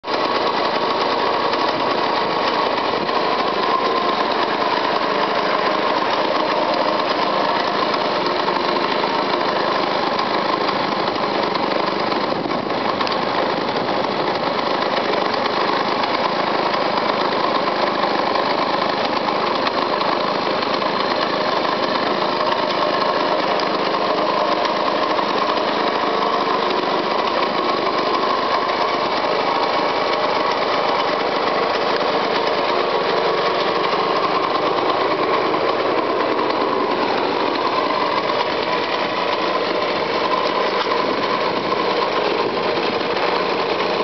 Teesside Yesteryear Motor Club175 Puch SVS runs up 14-11-09.AVI
1960 Puch 175cc SVS rebuilt 2007, running again after crap fuel burnt out pistons.